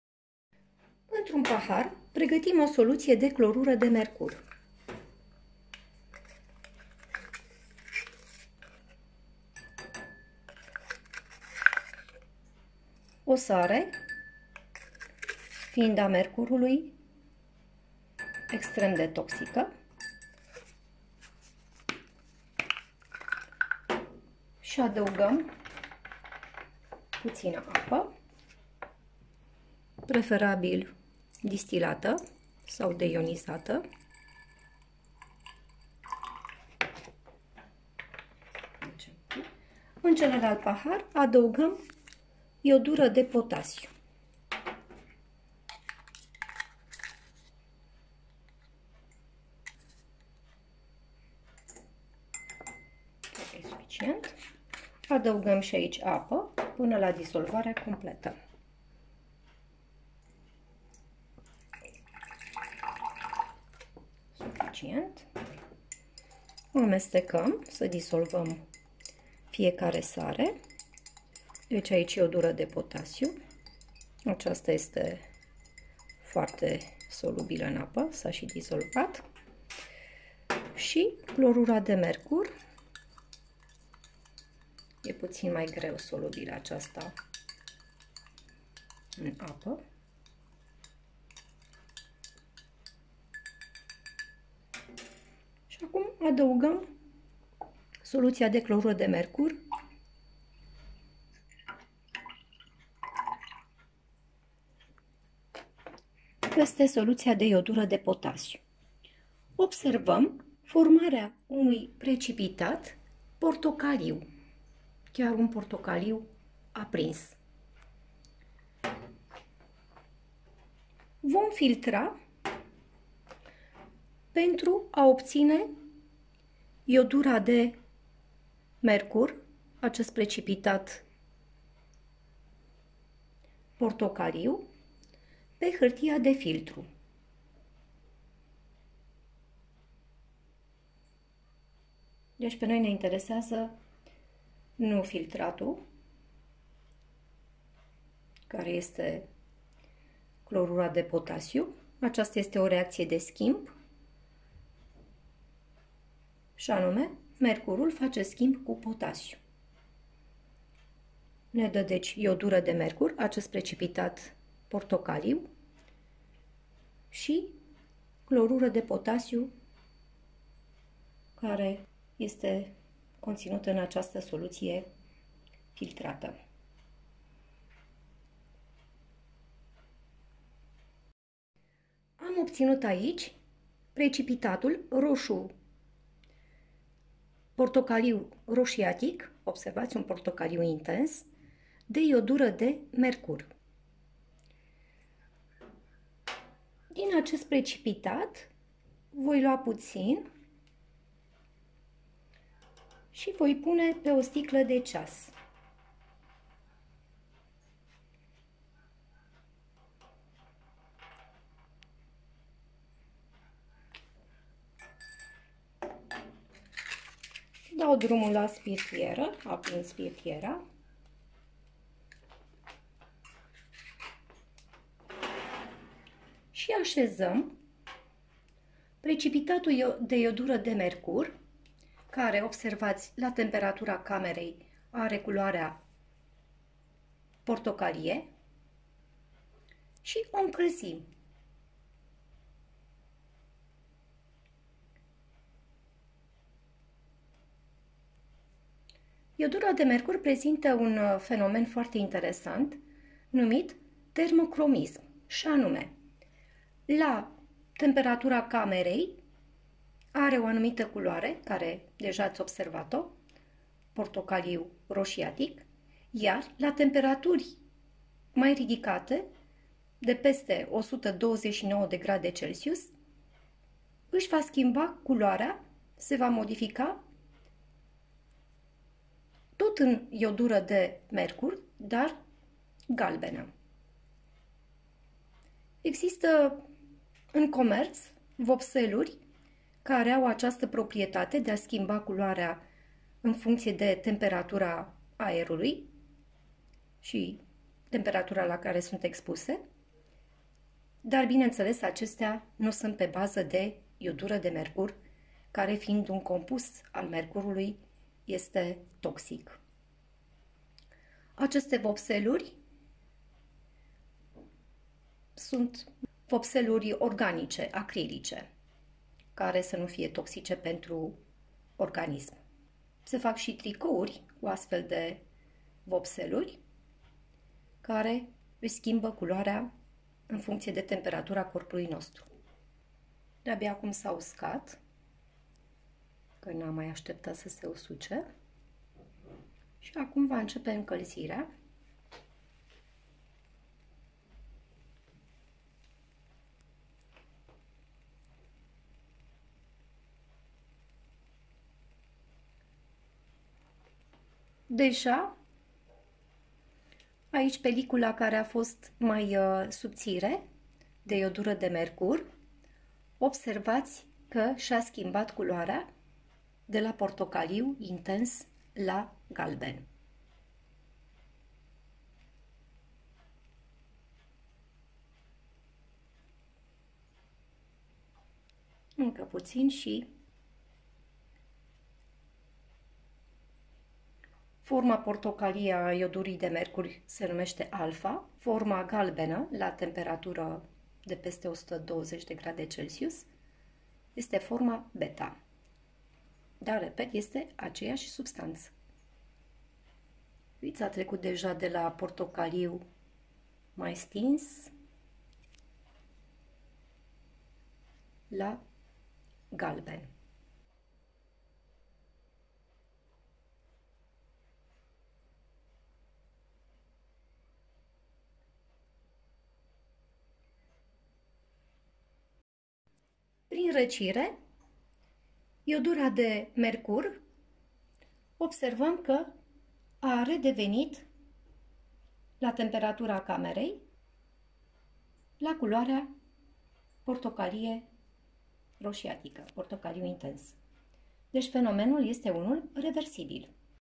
Reacția de schimb dintre clorura de mercur și iodura de potasiu - Termocromismul iodurii de mercur
Acest experiment se efectuează numai de către profesori! Clorura de mercur este extrem de toxică. Atenție când lucrezi cu surse de foc! - Fizica și Chimia pe înțelesul tuturor!